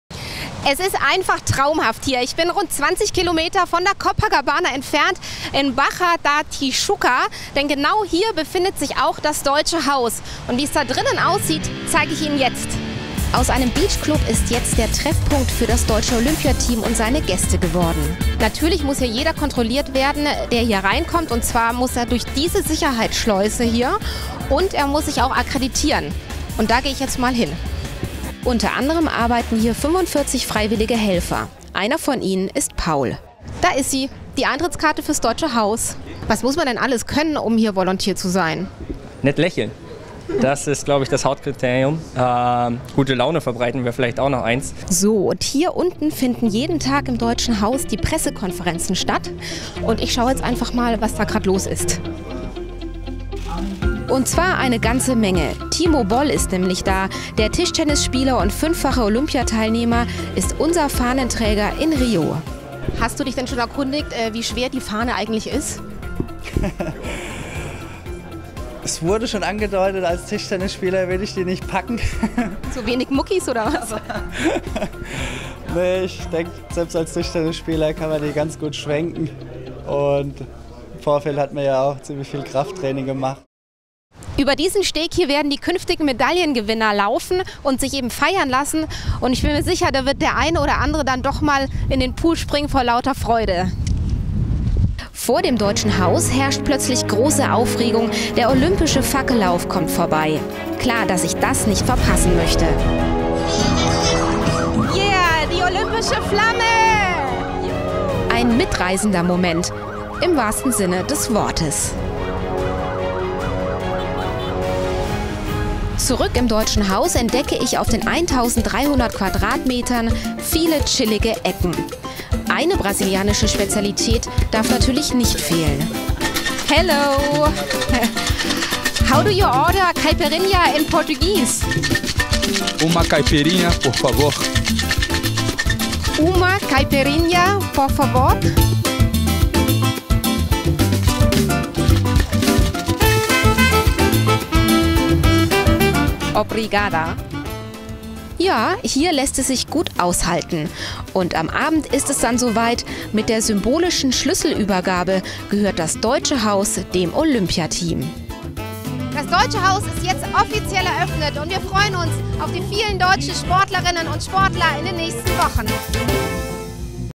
Rundgang im Deutschen Haus
Wie sieht es aus im Deutschen Haus? Ich schaue hinter die Kulissen und treffe auf Timo Boll: Mein erster Beitrag für n-tv!